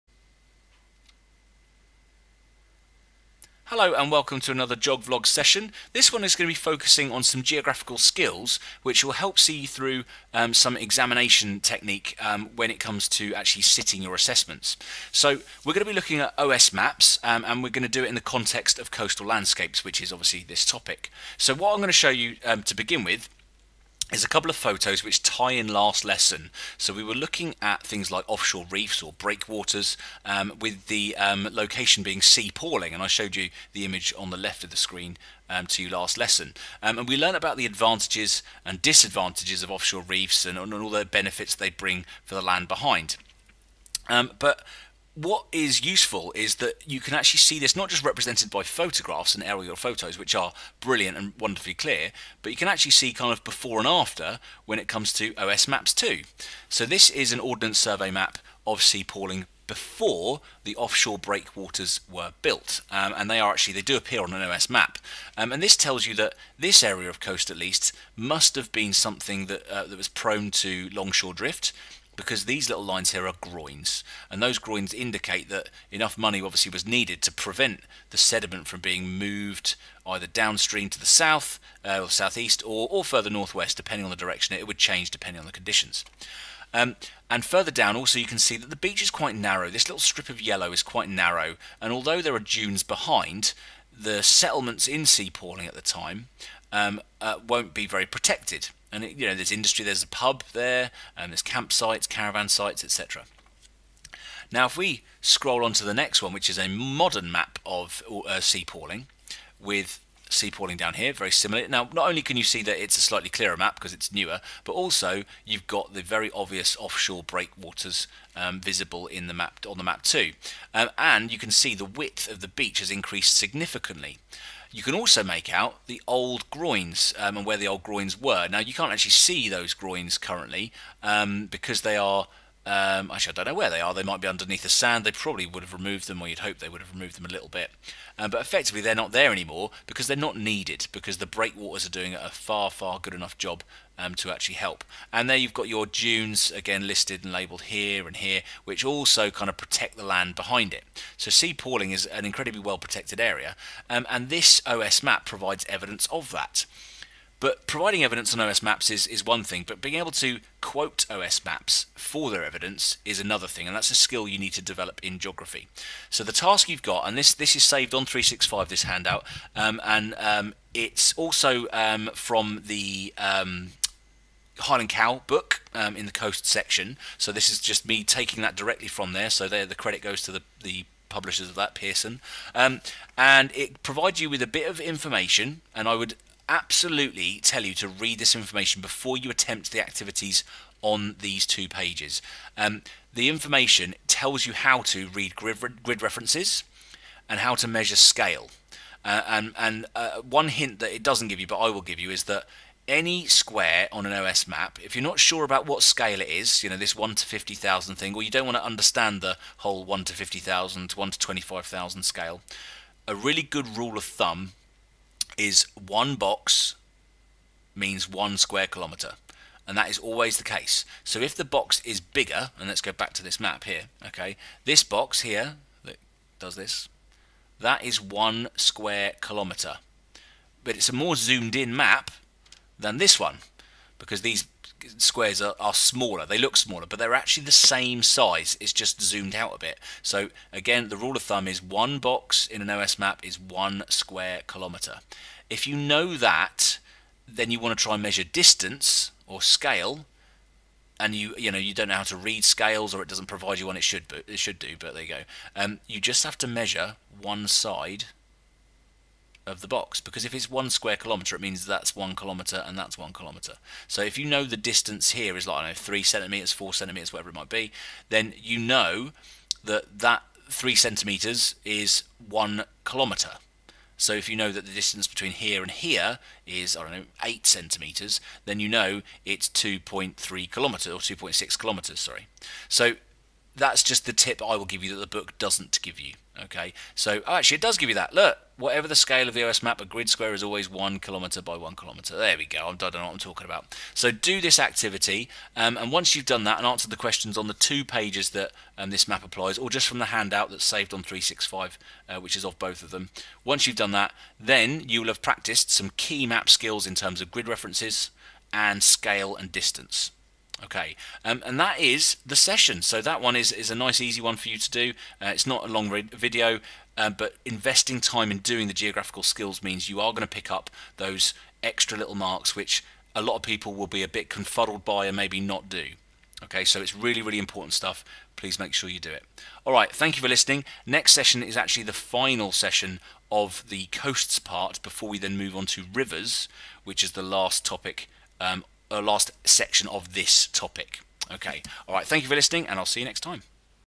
Changing UK Landscapes 11 - Coasts 8
This session is a skills based lesson where pupils can practice grid references and measuring distances on OS maps